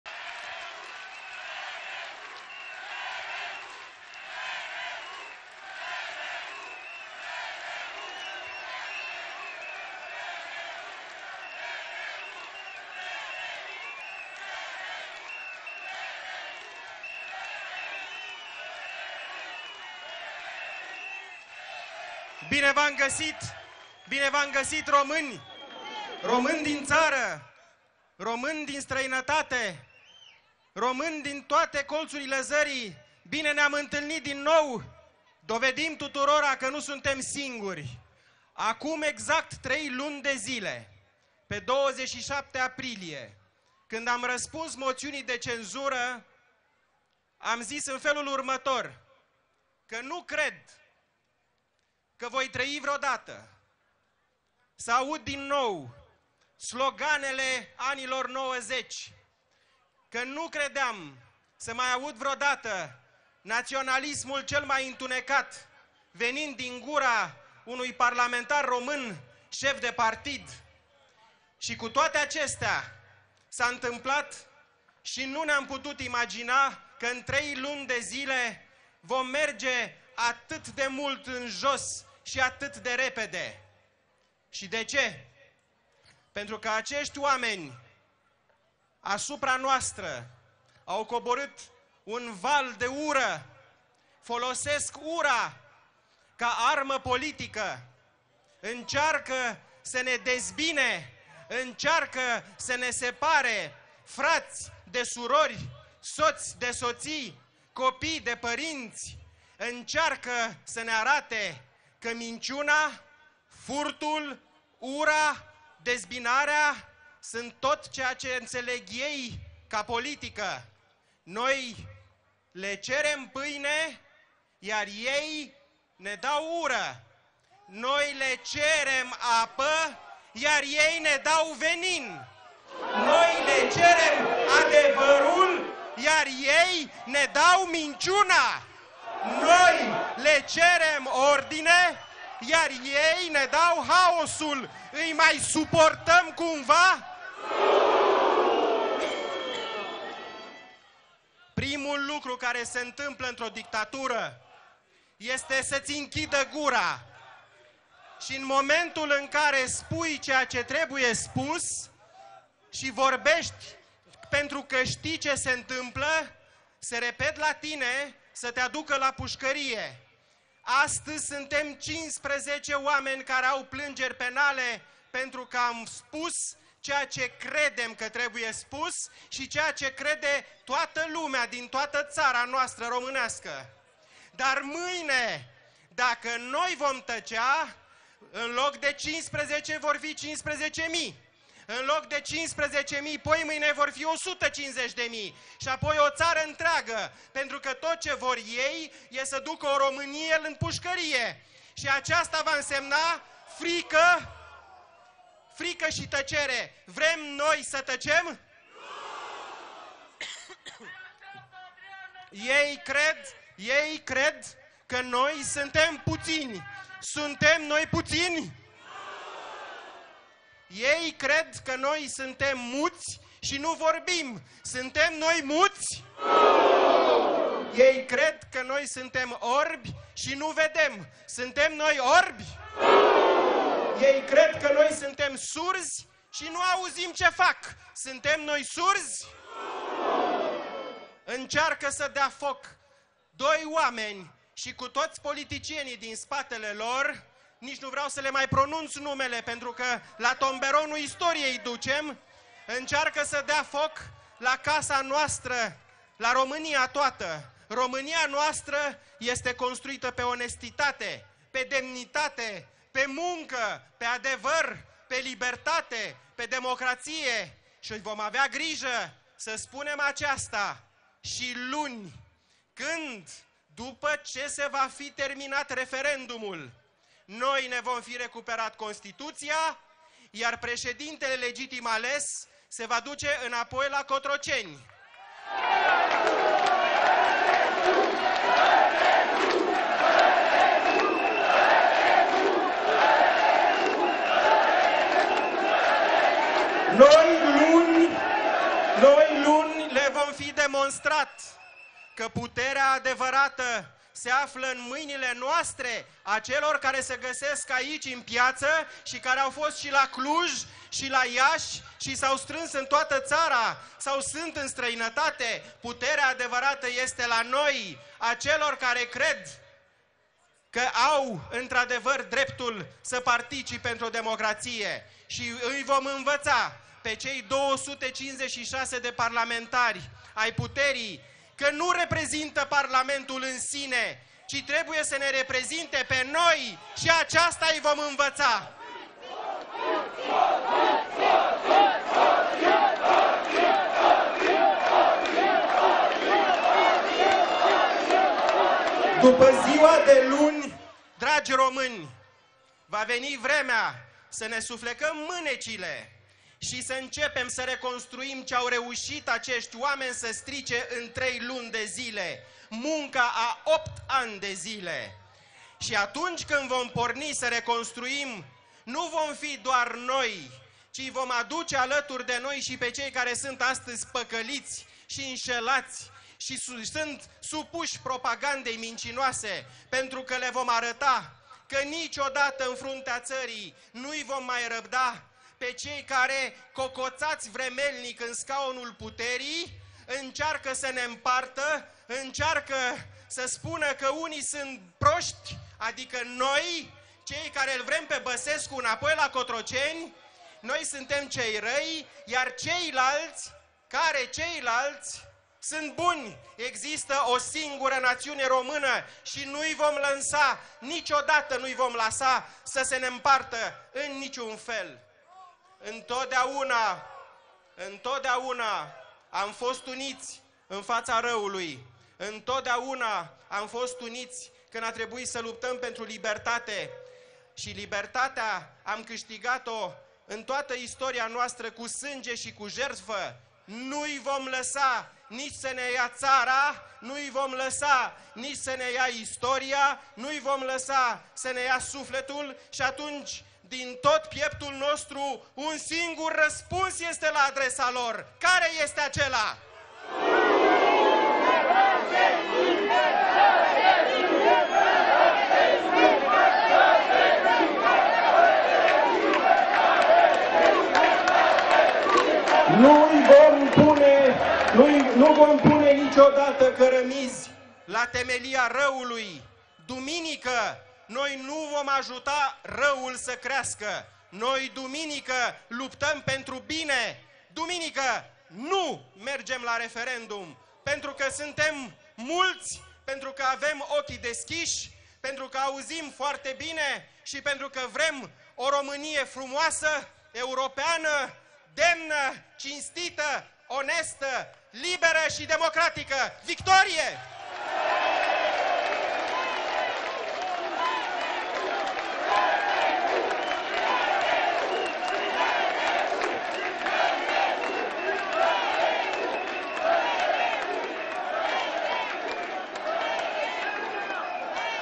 Mihai-Razvan Ungureanu la mitingul din Piata Revolutiei, 26.07.2012
Sursa: B1 TV